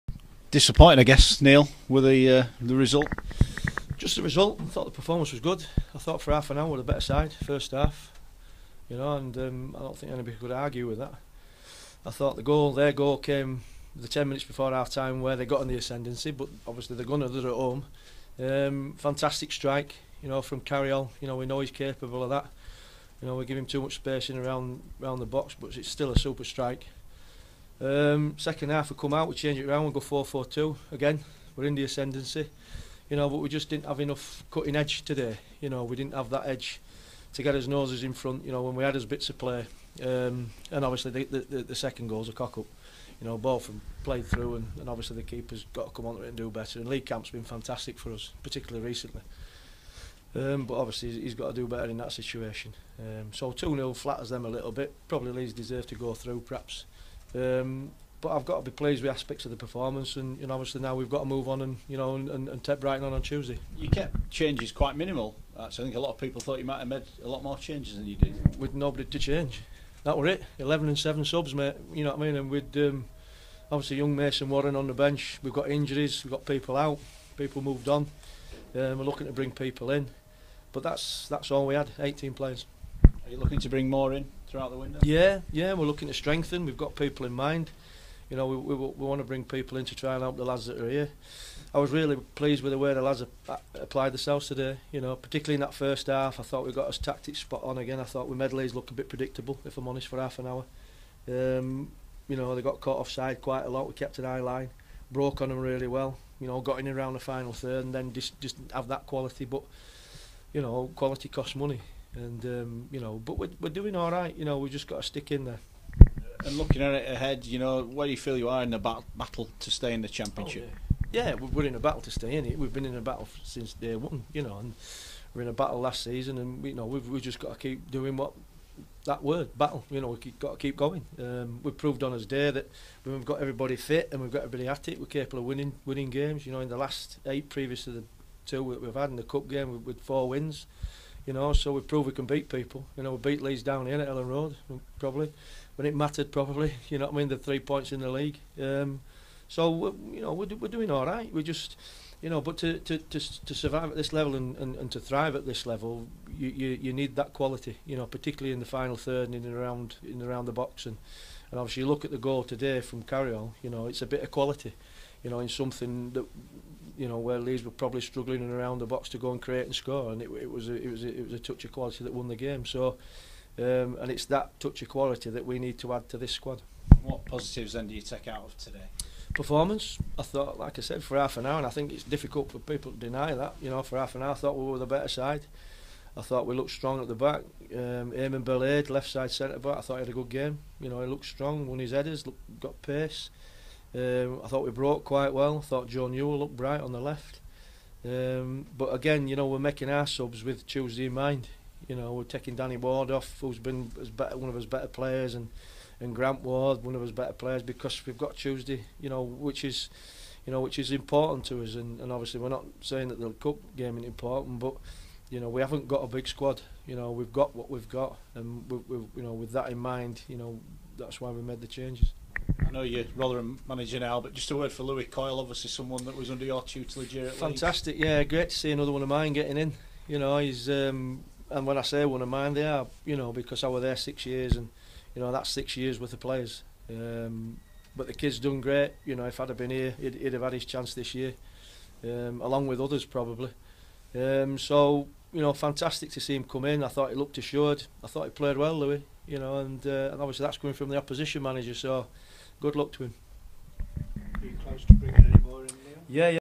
Neil Redfearn following 2-0 loss to Leeds United in the FA Cup
Rotherham boss Neil Redfearn reflects on a 2-0 defeat that saw the Millers exit the FA Cup in Round 3.

Goals for Carayol & Doukara saw Leeds United make it 8 games unbeaten